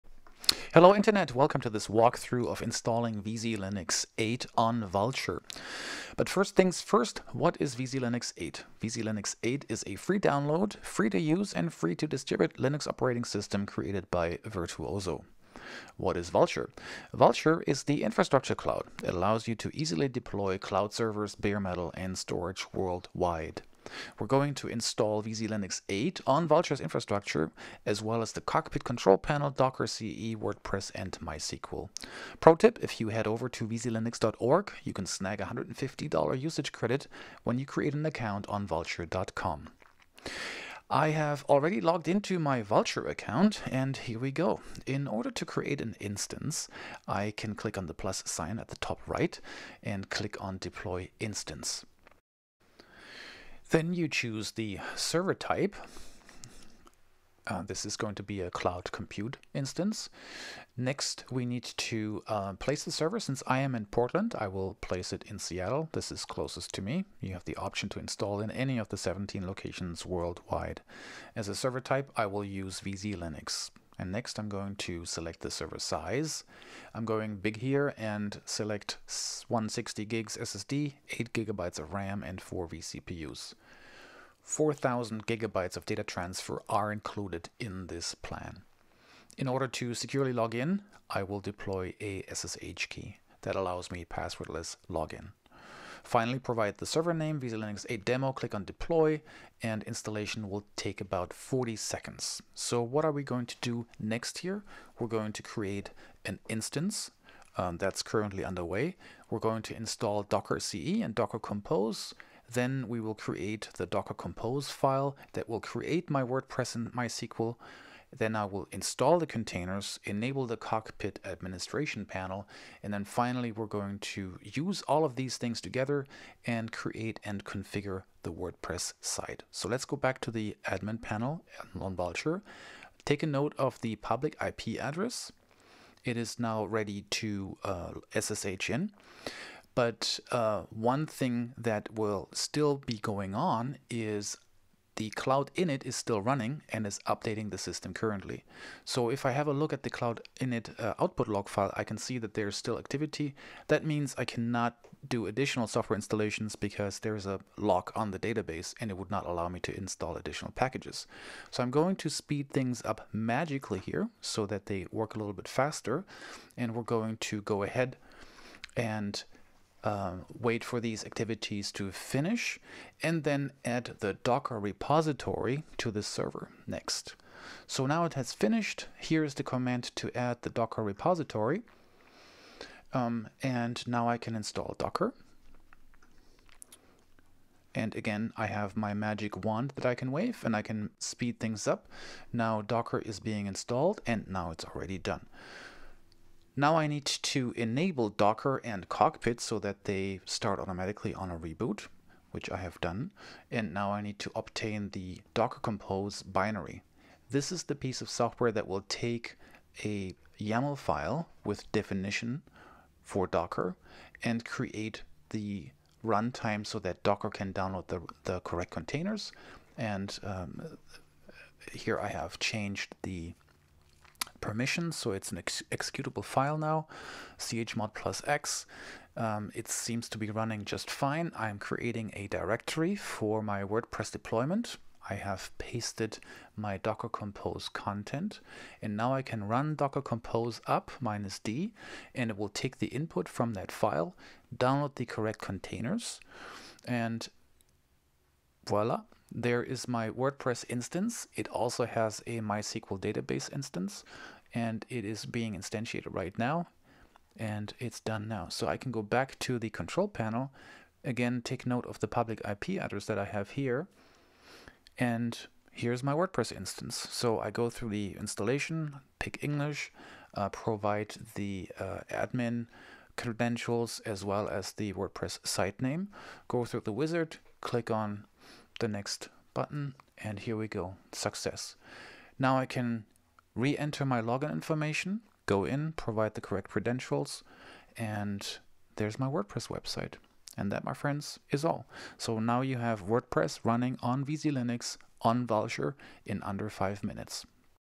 VzLinux 8 on Vultr
Watch as we install VzLinux 8 on Vultr. Simple deployment in minutes.